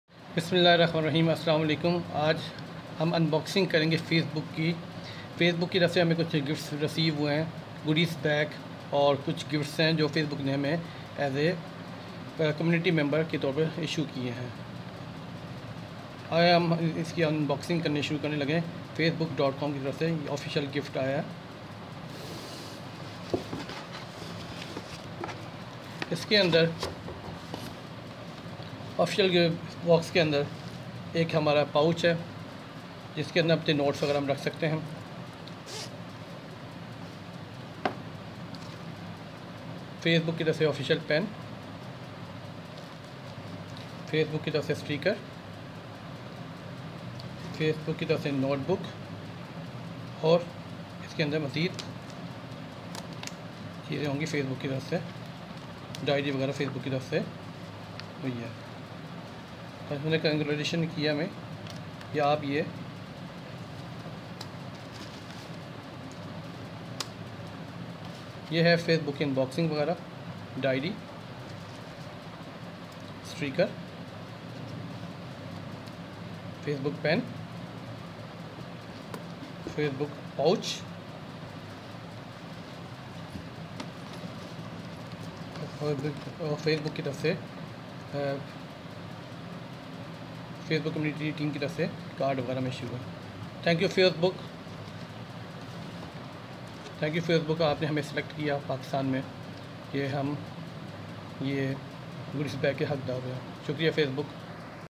Facebook Gift Unboxing | How to get gift from Facebook | Meta Gift Unboxing
Every day, millions of people share special moments with their friends on Facebook by saying “Happy Birthday,” “Congratulations,” or simply, “I’m thinking of you.” Now there is another way to celebrate those moments.

Starting today, you can give real gifts to your friends on Facebook using Gifts.

Facebook wants to help other businesses sell things, so it’s done selling its own. With Gifts on its way out, Facebook can focus on what does work: helping businesses sell more to Facebook’s users, rather than selling products to them directly. Instead of replacing their stores, Facebook just wants to smooth the checkout flow, show ads that lead to purchases, and prove those ads work.